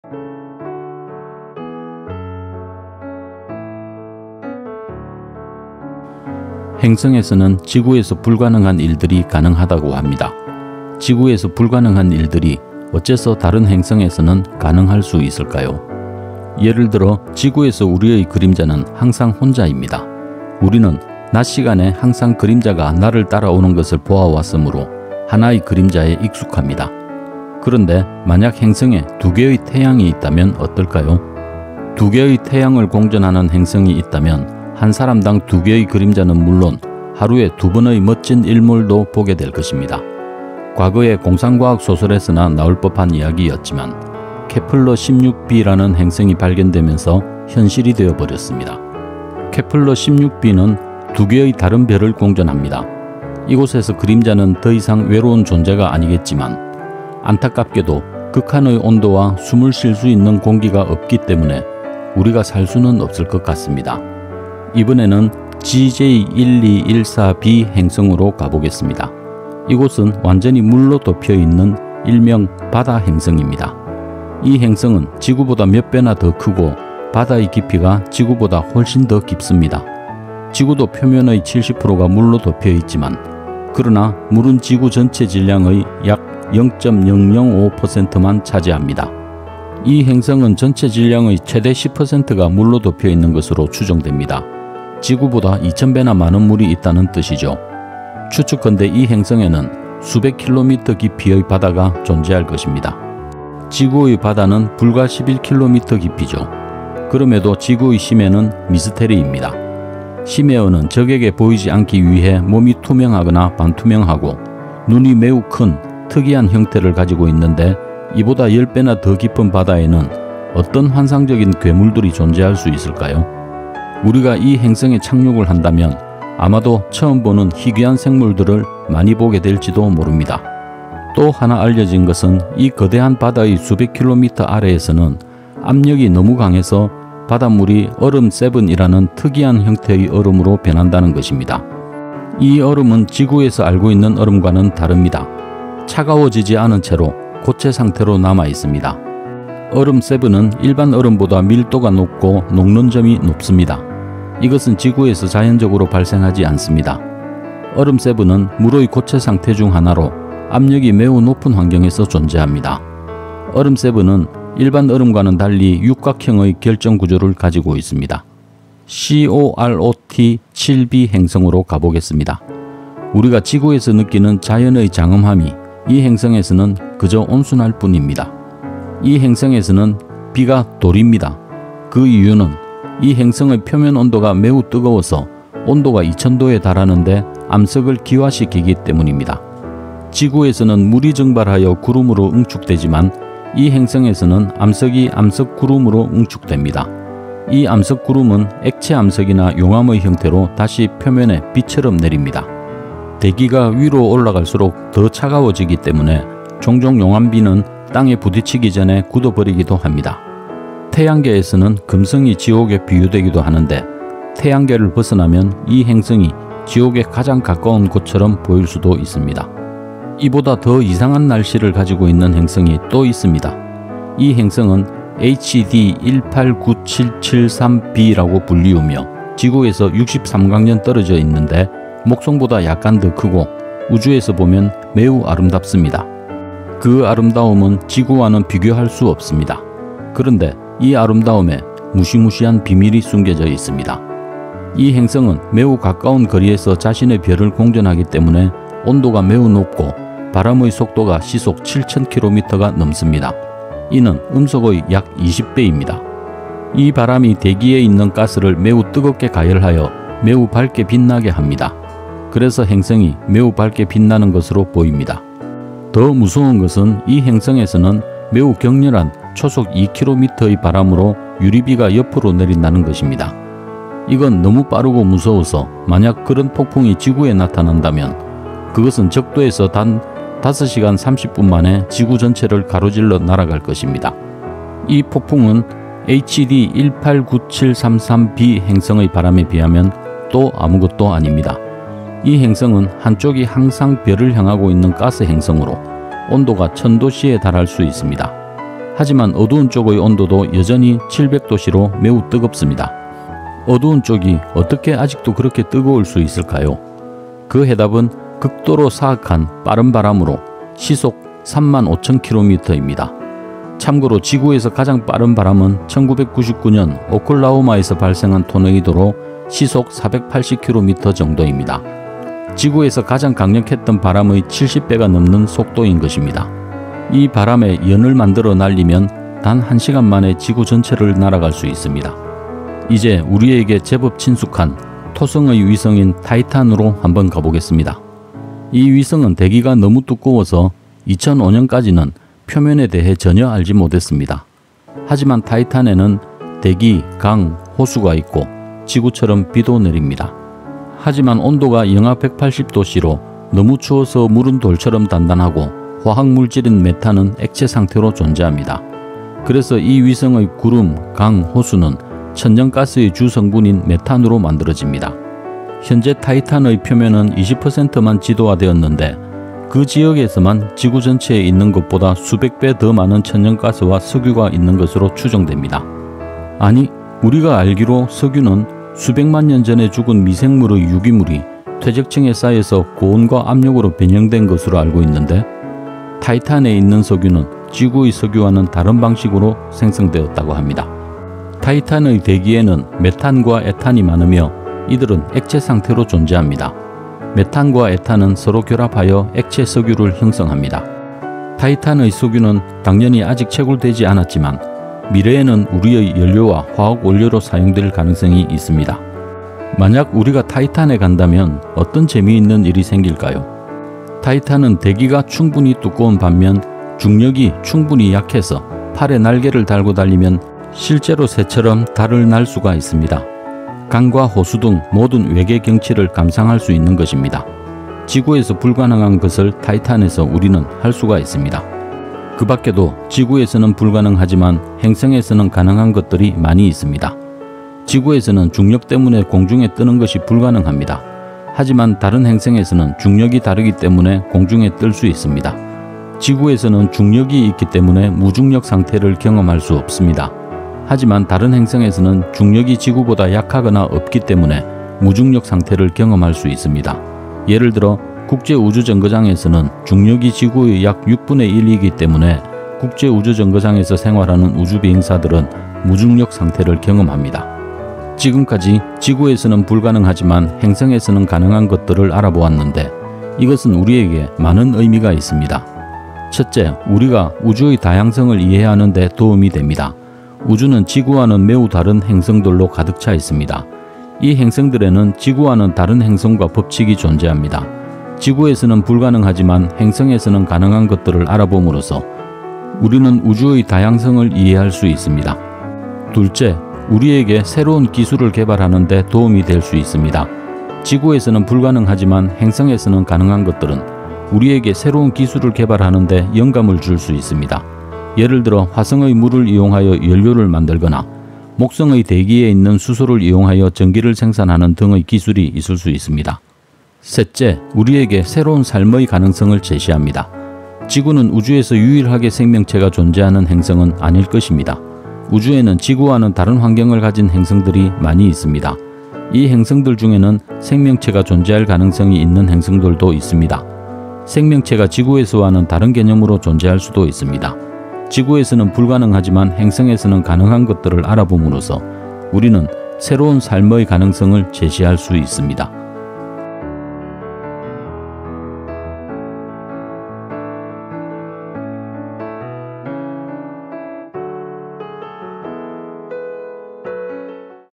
우주 행성 탐험! 지구에서는 상상할 수도 없지만 다른 행성에서는 버젓이 일어나는 흥미로운 자연현상들, 이곳에서 외계인이 존재할 수 있을까?
지구에서는 안되지만 다른 행성에서는 가능한 것들이 많이 있습니다. 이 영상에서는 지구와 다른 행성의 특징을 비교하고, 지구에서는 안되지만 다른 행성에서는 가능한 재미있는 현상들을 소개합니다. 대부분의 내용은 과학적인 관점에서 확인된 사실입니다
이 영상의 목적은 우주의 다양성을 이해하고, 새로운 기술개발의 영감을 얻고, 우리에게 새로운 삶의 가능성을 얻기 위함입니다

National Aeronautics and Space Administration, Public domain, via Wikimedia Commons